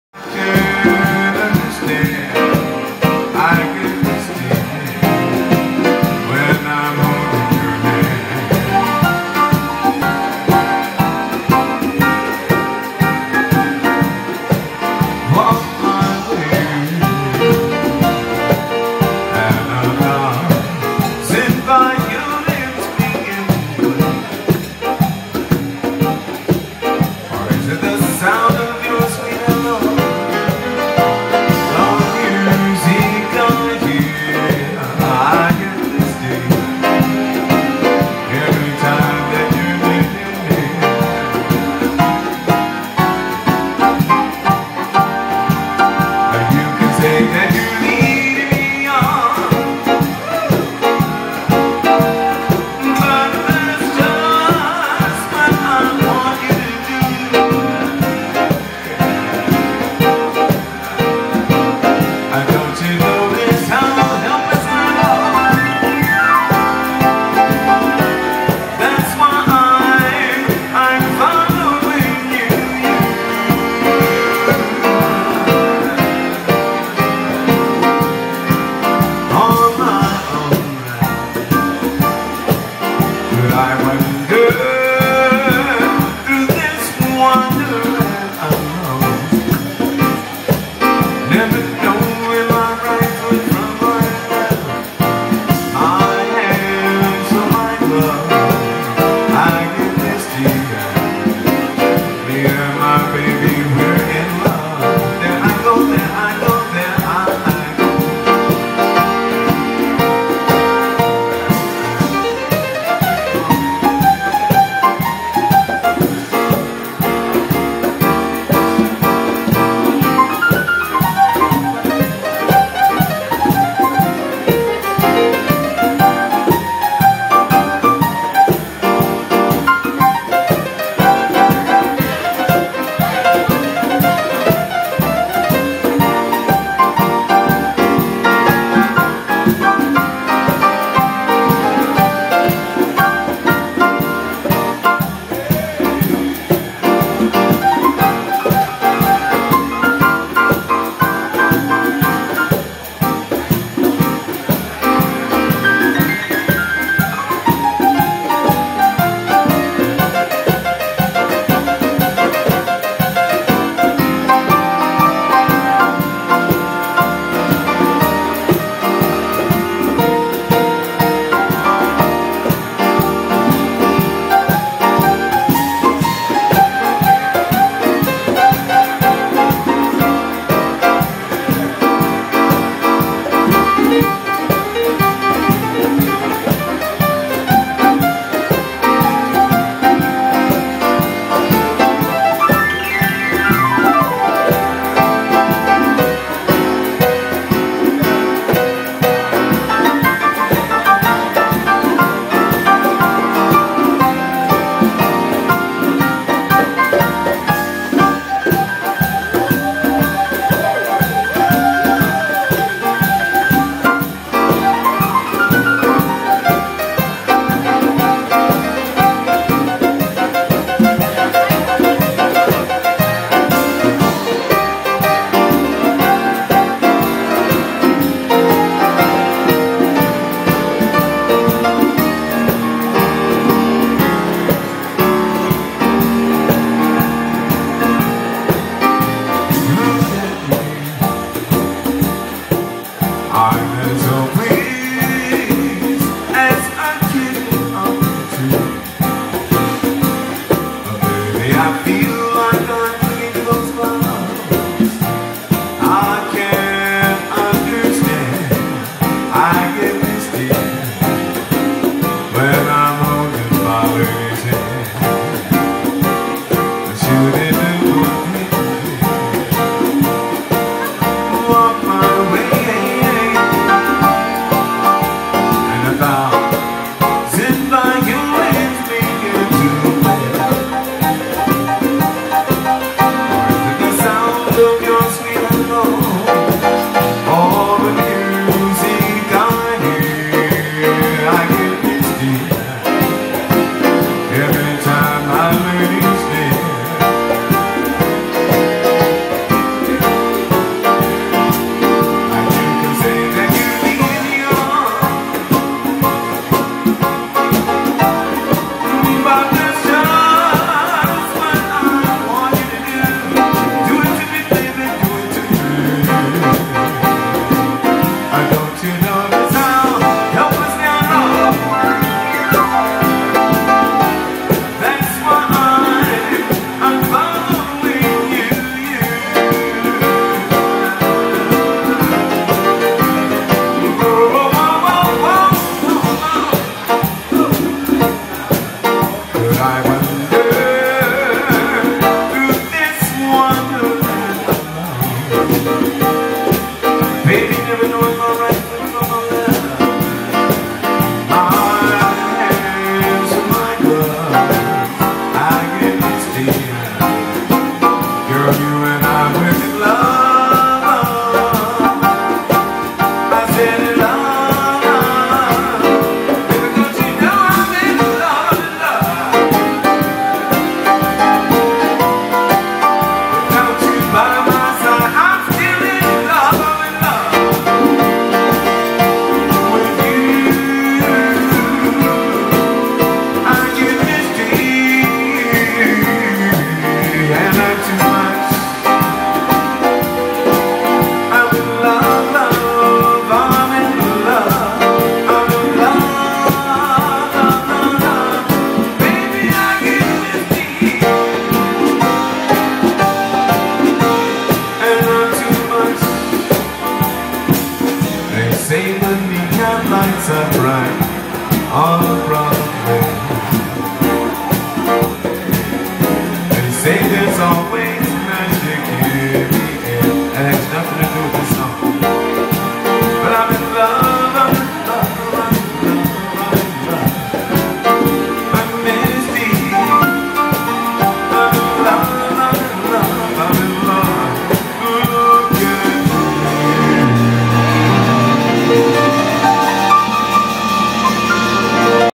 Ghalib Ghallab
Ghalib Ghallab performing at the Range Steakhouse in Harrahs Hotel
featuring Jihad Ghallab (drums) and
Tony Rodriguez (congas)
New Years Eve 1 /1/11